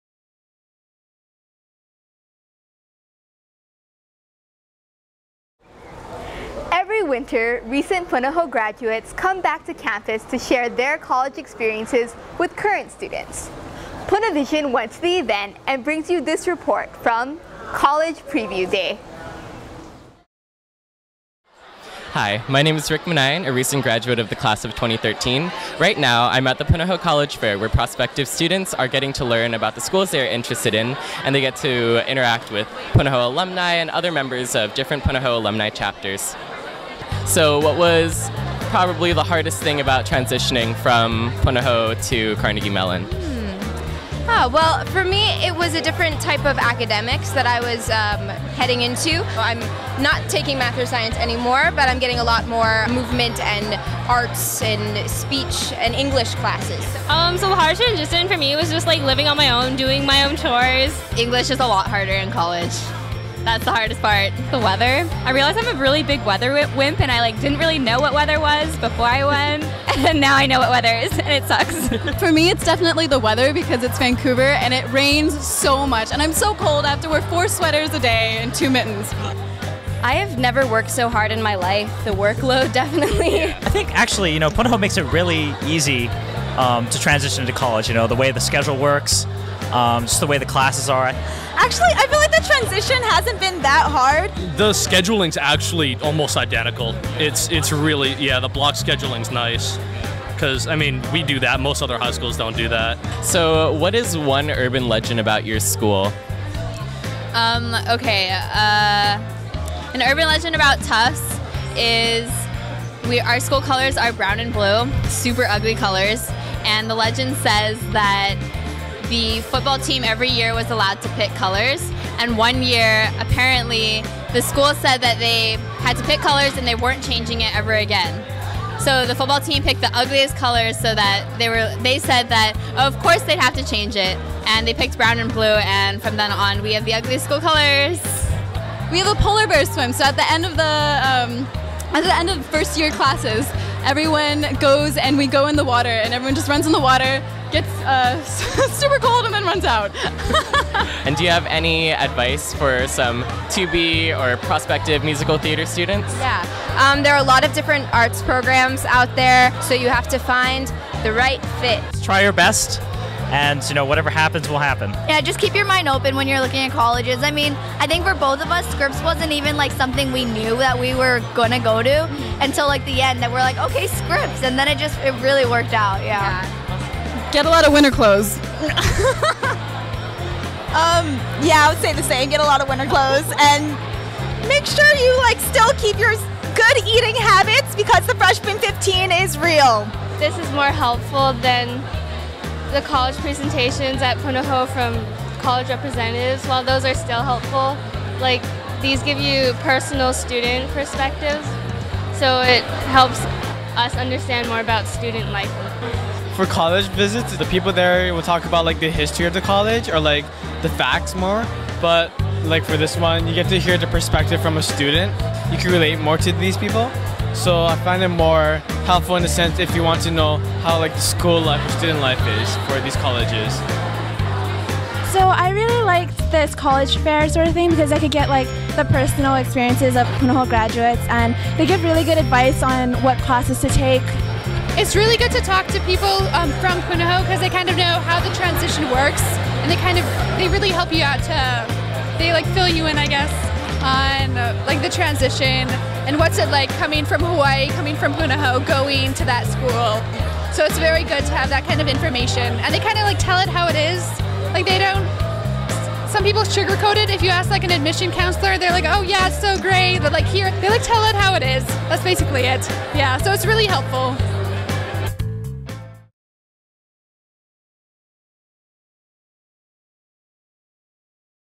College Preview Day (Punavision - February 2014)
Every winter, recent Punahou graduates come back to campus to share their college experiences with the current students.  Punavision went to the event and brings you this report from College Preview Day.

Produced & edited by Katja Kwaku '16